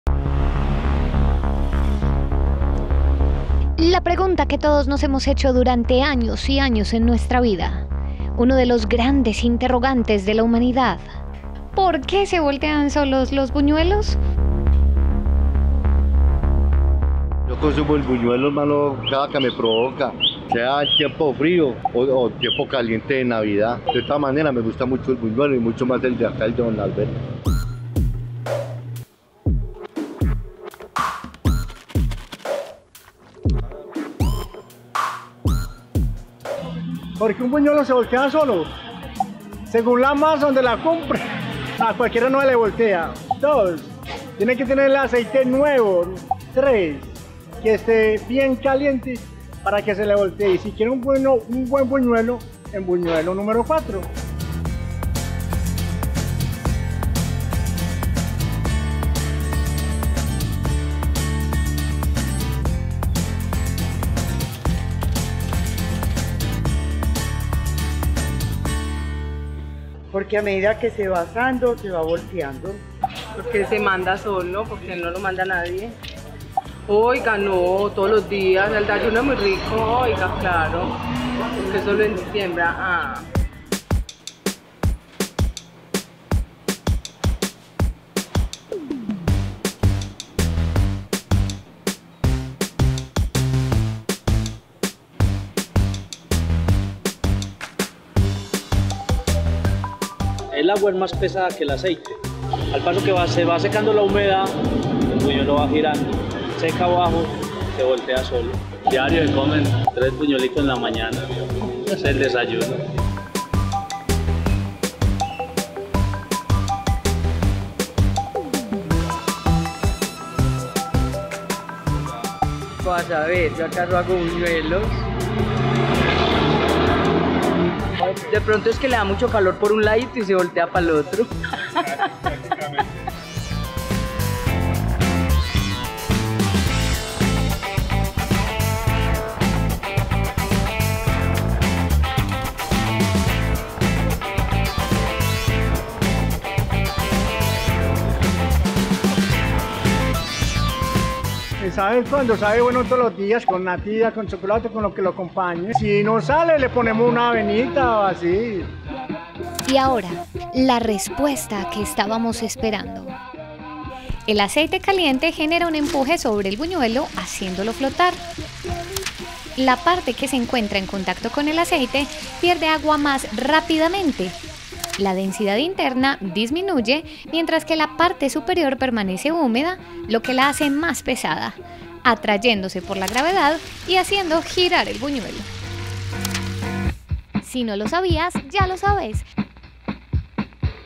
Por qué los buñuelos se voltean solos
Pareciera que hacer un buñuelo no tiene ninguna ciencia, pero detrás de algo tan cotidiano, existen un montón de condiciones físicas y  químicas. Pero ¿sabe usted por qué los buñuelos se voltean solos? Aquí en Versión  Beta, se lo contamos.